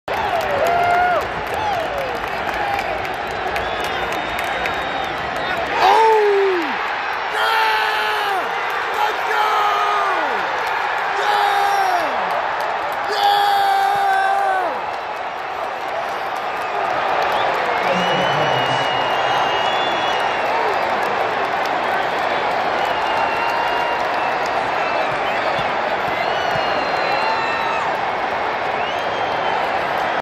Daniel Jones touchdown pass to Darius Slayton vs New York Jets at MetLife Stadium
New York Giants quarterback Daniel Jones throws a slant pass to wide receiver Darius Slayton for a 39 yard touchdown pass vs the New York Jets on November 10th, 2019 in the MetLife Bowl.